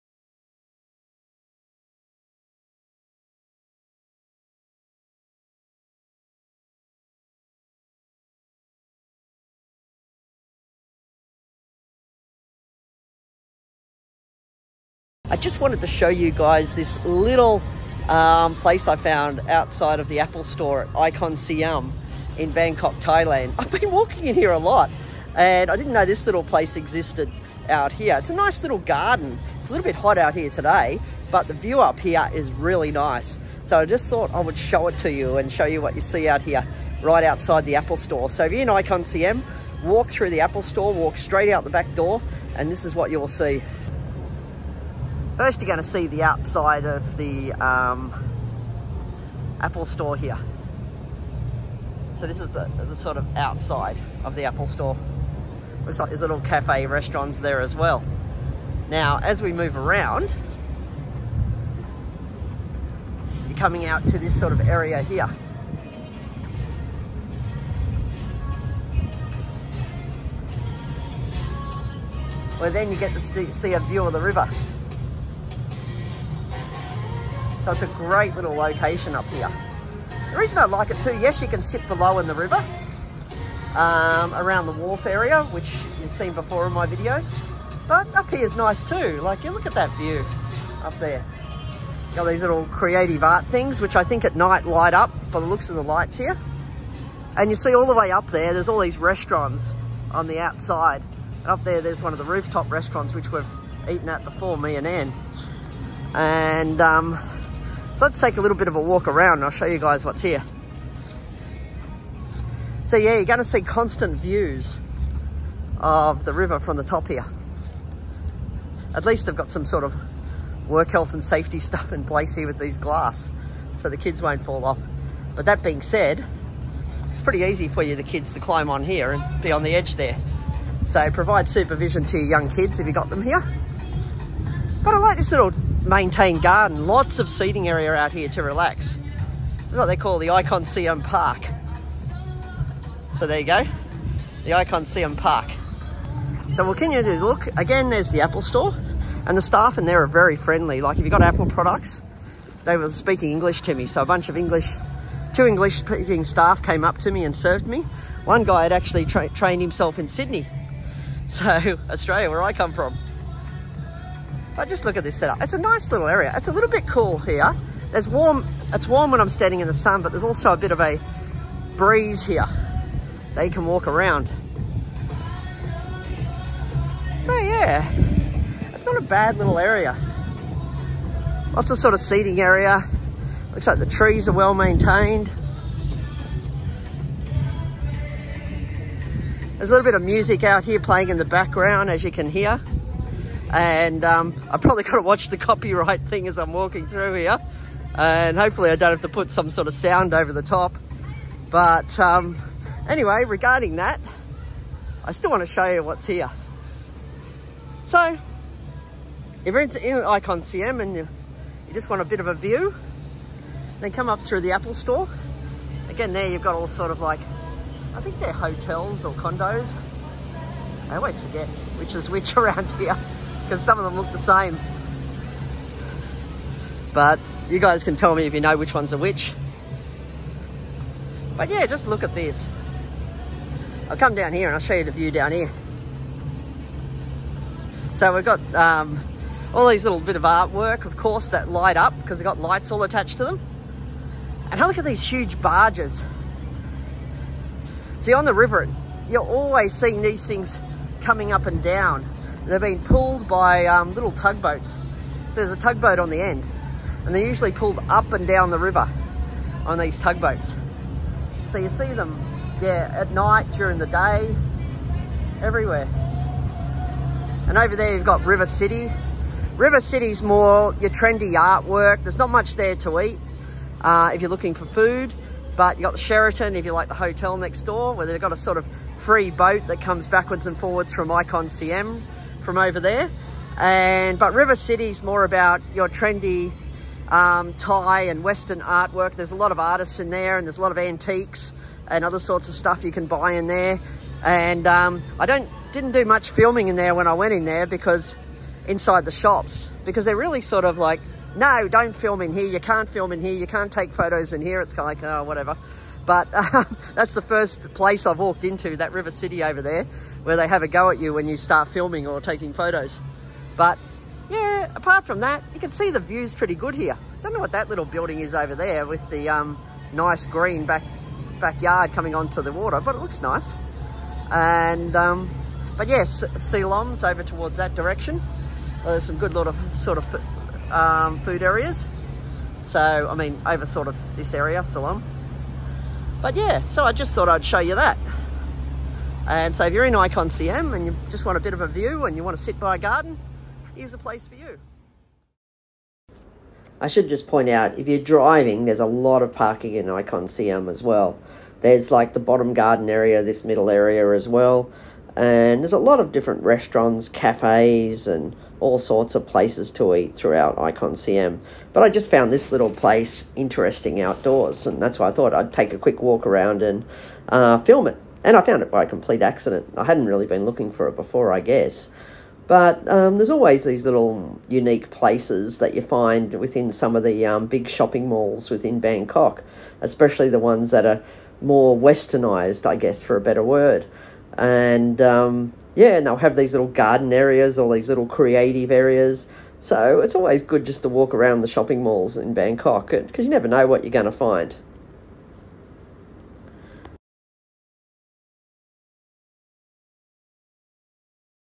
Great little place to relax outdoors in ICONSIAM shopping mall Bangkok Thailand
Great little place to relax outdoors in ICONSIAM shopping mall in Bangkok Thailand - ICONSIAM Park